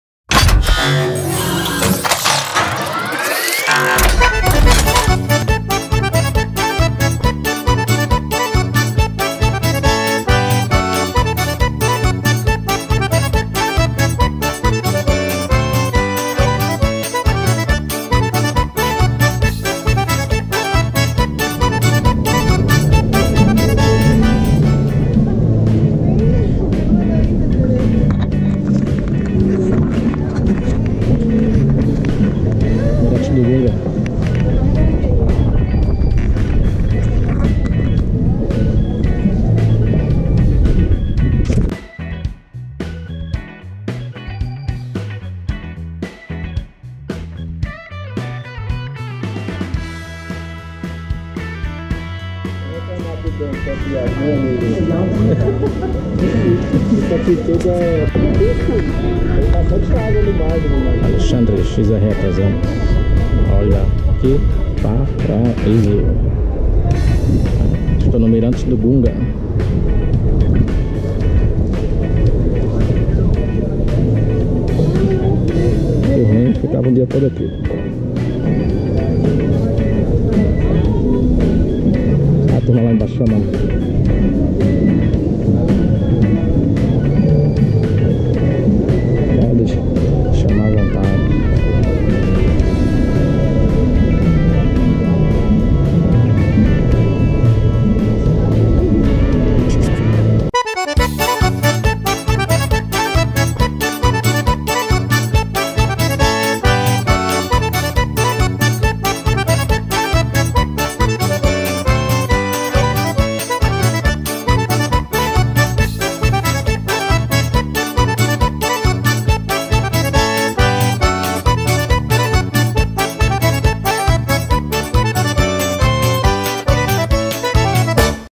Mirante do Gunga - EP-07/07 -XRE300
Neste último vídeo da série "Penedo Motofest" fui até o mirante do Gunga o qual conheci pelo canal BodesWell.com no facebook e site quando passaram pelo Brasil em sua mega jornada.
I met another paradise, the Gunga viewpoint.

XRE300 2013

LaranjaMecânica
Alien Motos
Tasouza Rider
Queiroz_zs